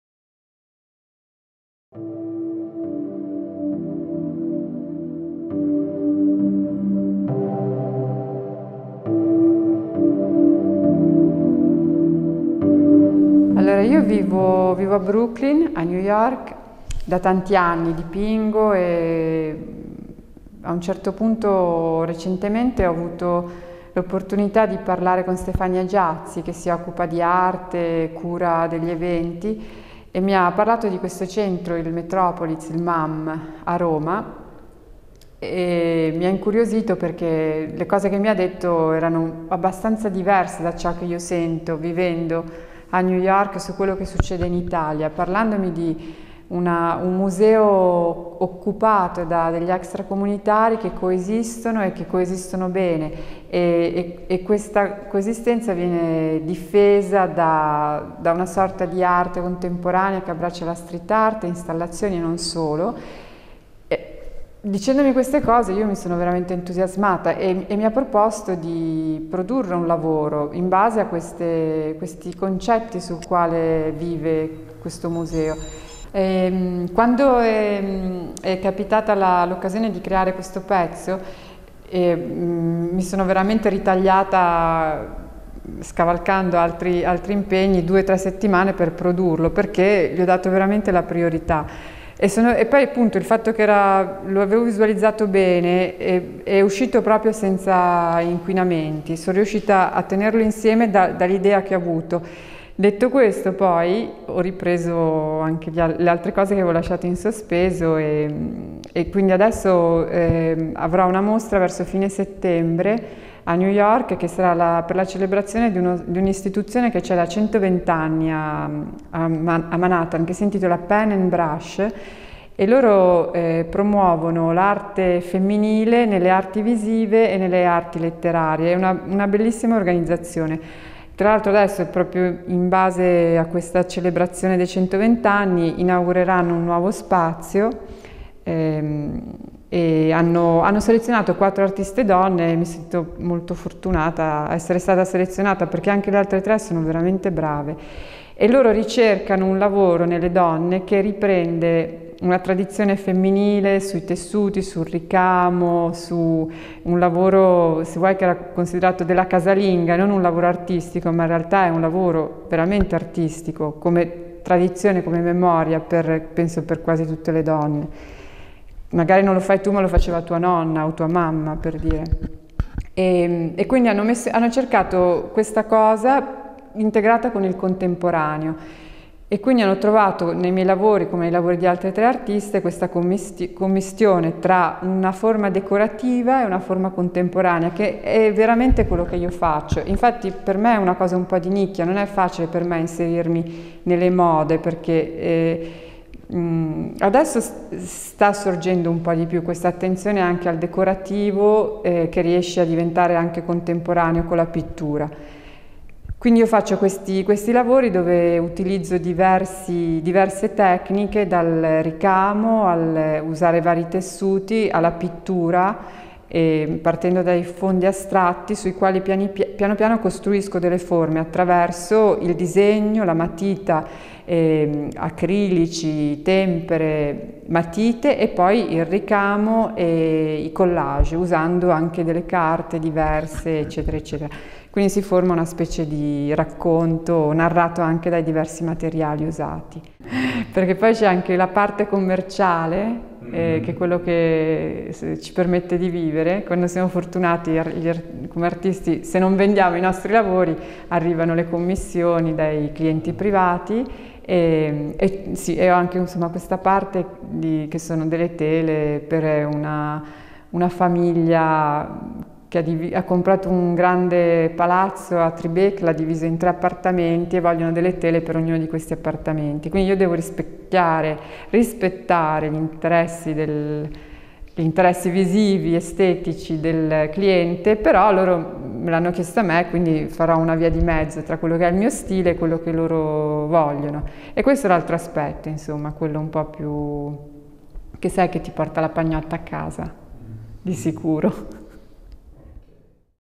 Incontro al Maam con l'artista Michela Martello
“I miei lavori sono questa commissione tra una forma decorativa e una forma contemporanea astratta. Non è facile per me inserirmi nelle mode ma adesso sta sorgendo un po’ di più di quest’attenzione al decorativo che riesce a diventare anche contemporaneo con la pittura Quindi, realizzo questi lavori dove utilizzo diverse tecniche dal ricamo all’uso di varie tessuti alla pittura e partendo dai fondi astratti piano piano costruisco delle forme attraverso il disegno con la matita, acrilici, tempere e poi il collage usando anche delle carte diverse quindi si forma una specie di racconto narrato anche dai diversi materiali usati. ”